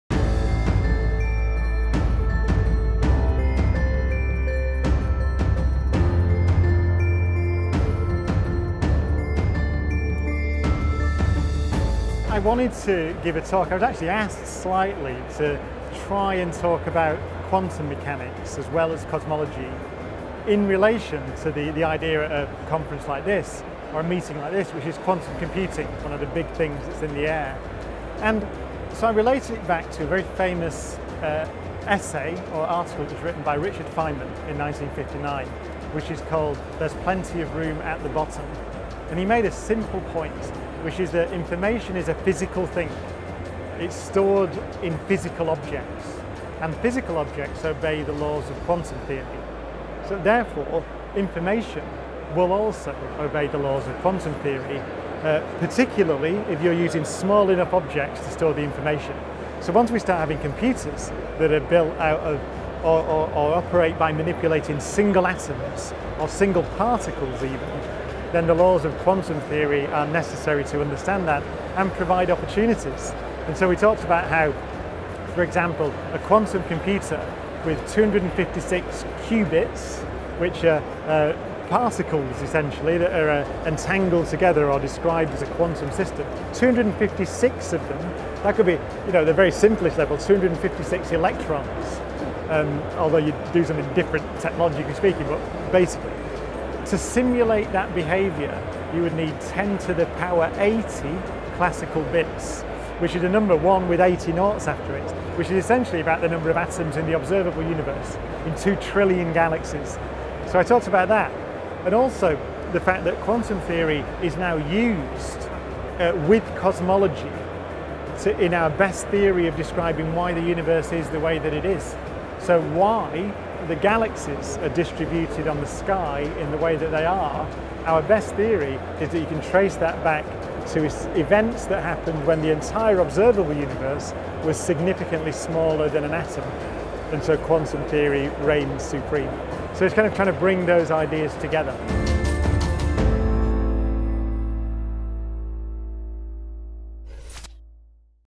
Professor Brian Cox - Part 1 – Professor Brain Cox at IP EXPO Europe 2017
Professor Brian Cox discusses his opening keynote seminar at IP EXPO Europe 2017 about Quantum Mechanics as well as Cosmology in relation to Quantum Computing. Professor Brian Cox poses a physicist’s take on technology with his trademark infectious enthusiasm that makes science fun and life-changing